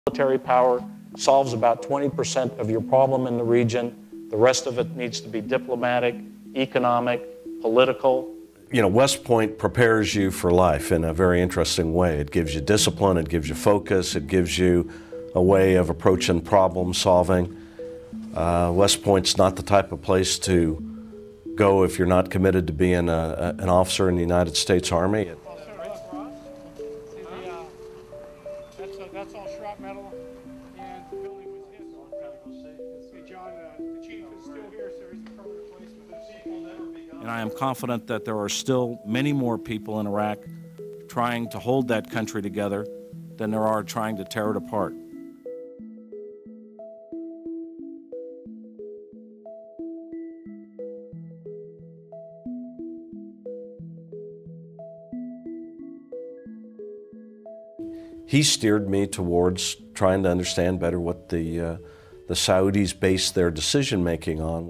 Who is John Abizaid?
Retired Army Gen. John Abizaid, who is best known from his time overseeing the Iraq War, is expected to become the U.S. ambassador to Saudi Arabia. If confirmed, he would likely play a key role in the Trump administration’s response to the killing of Washington Post contributor Jamal Khashoggi at the Saudi consulate in Istanbul.